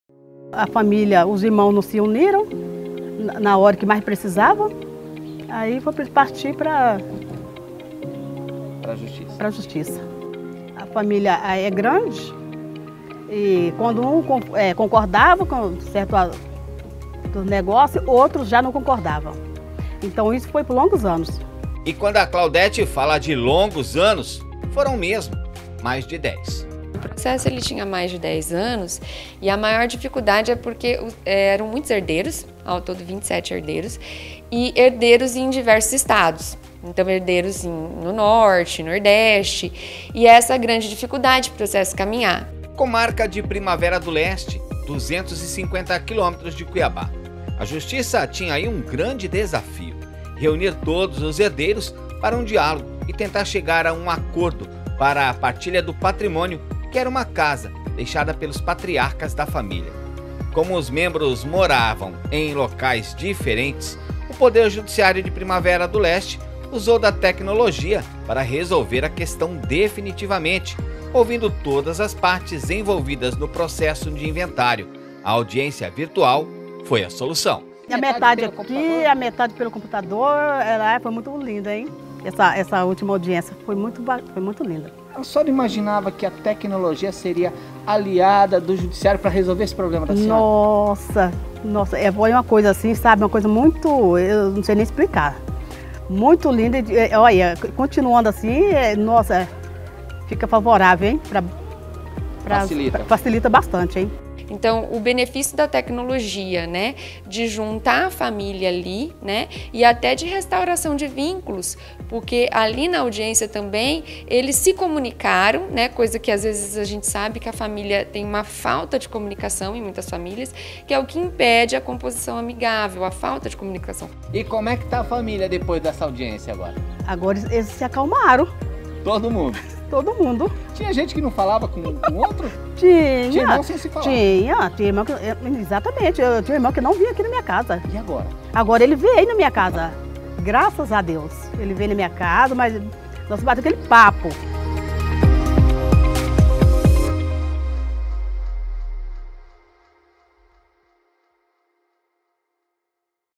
Judiciário realiza audiência virtual conectando partes em diversas localidades do Brasil
Com as audiências virtualizadas, a Vara de Família e Sucessões da Comarca de Primavera do Leste resolveu o impasse de um inventário que aguardava solução há 13 anos. Graças à tecnologia, a Justiça de Mato Grosso conseguiu reunir on-line os membros da família que residiam em diferentes localidades do país e entrar em um acordo.

Confira ▶️